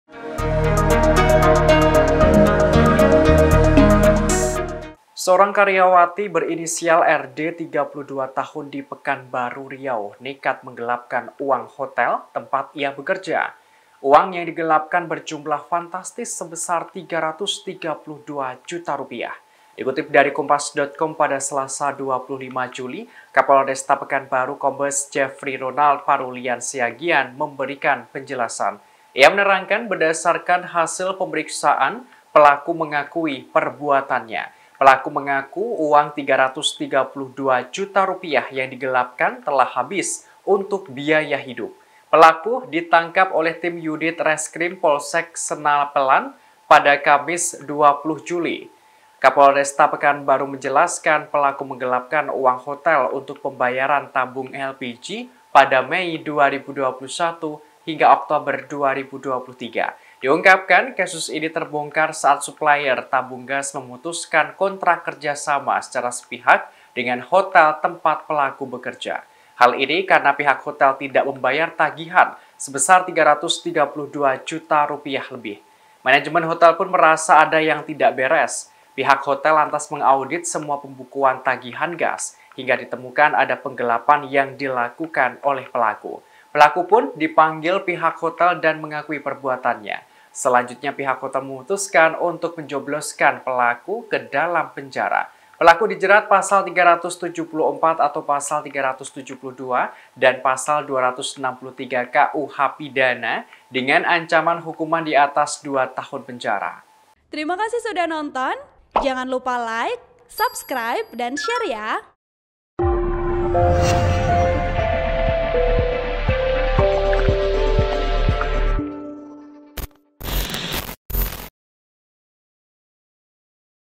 Karyawati Gelapkan Uang Hotel Rp 332 Juta Demi Biaya Hidup, Terbongkar saat Manajemen Audit Tagihan
TRIBUN-VIDEO.COM - Seorang karyawati berinisial RD (32) di Pekanbaru, Riau nekat menggelapkan uang hotel tempat ia bekerja.

Uang yang digelapkan berjumlah fantastis sebesar Rp 332 juta.

Dikutip dari Kompas.com pada Selasa (25/7/2023), Kapolresta Pekanbaru Kombes Jefri Ronald Parulian Siagian memberikan penjelasan.